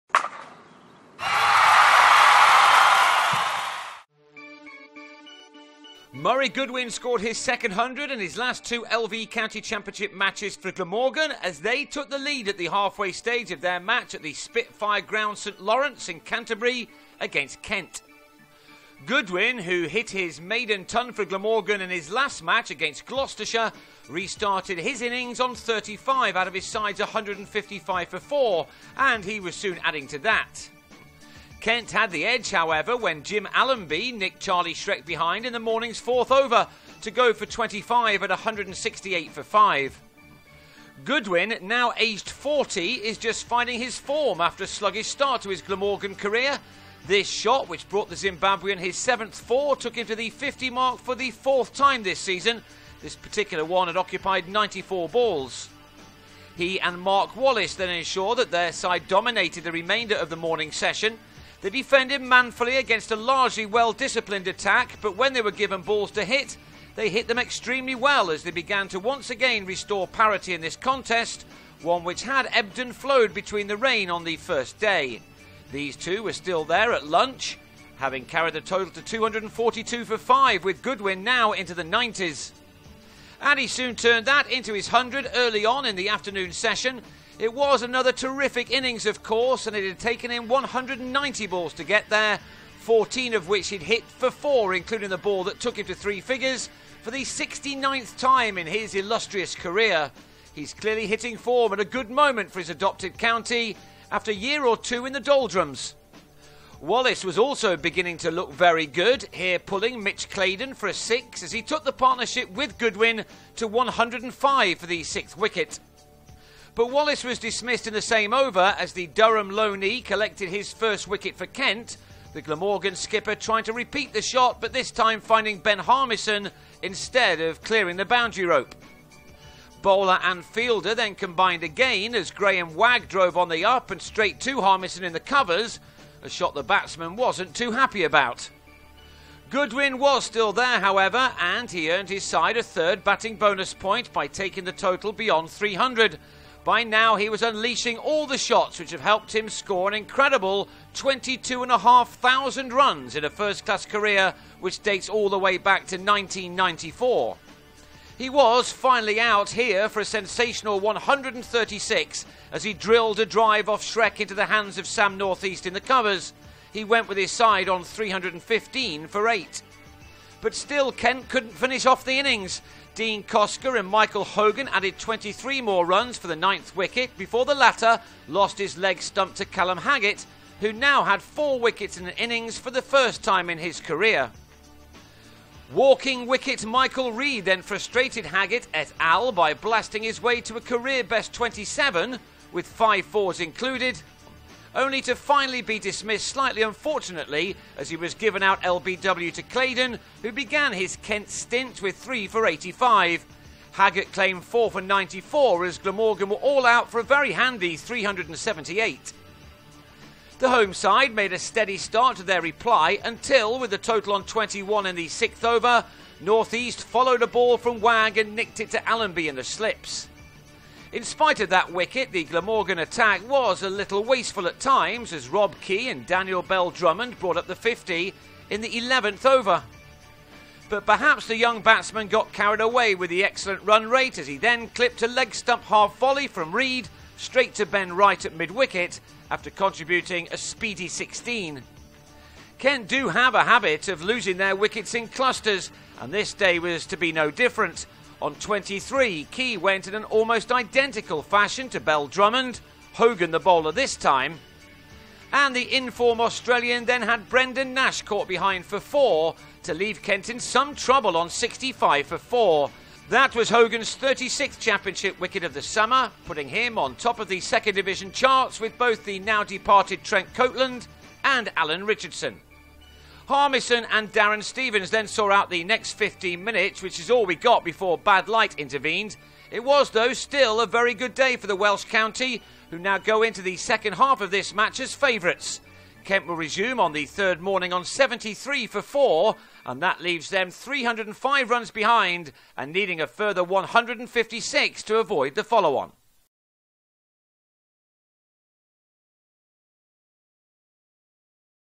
Kent v Glam day 2 highlights
Glamorgan continue to build a good first innings total at Canterbury before Kent start their reply in the LV= County Championship clash day 2